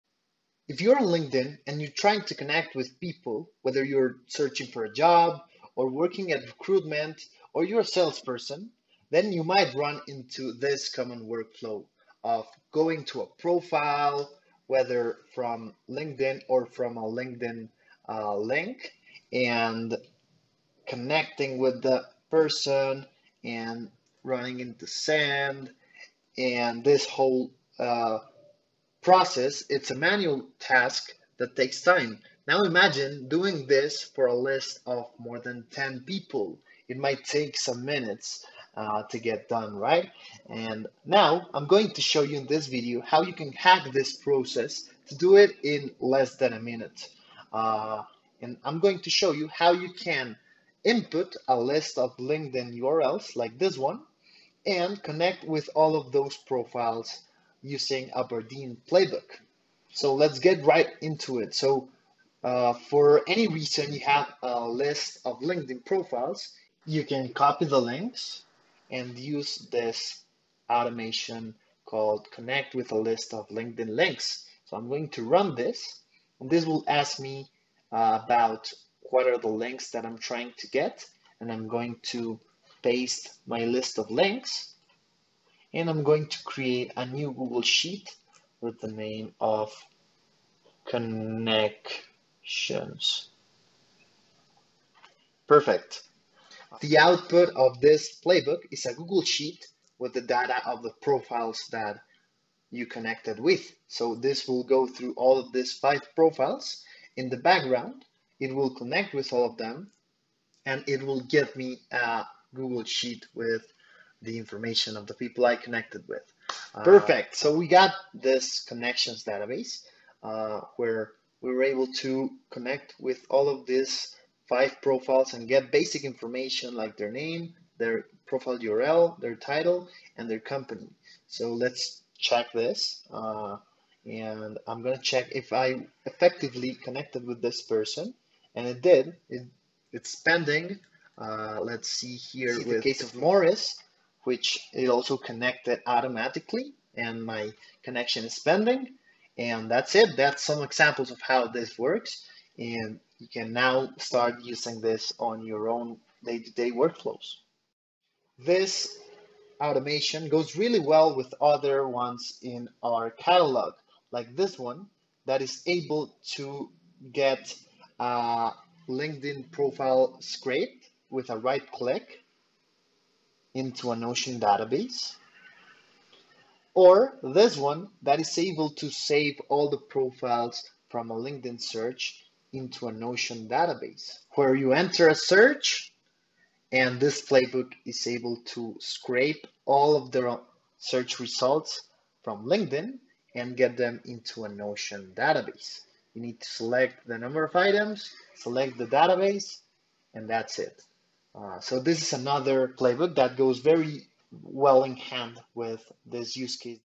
Connect to a list of LinkedIn contacts in seconds, not minutes
Save yourself the trouble of entering every profile to connect, do it automatically with this playbook. Just input a list of LinkedIn links and name the output google sheet, and let Bardeen do the work for you!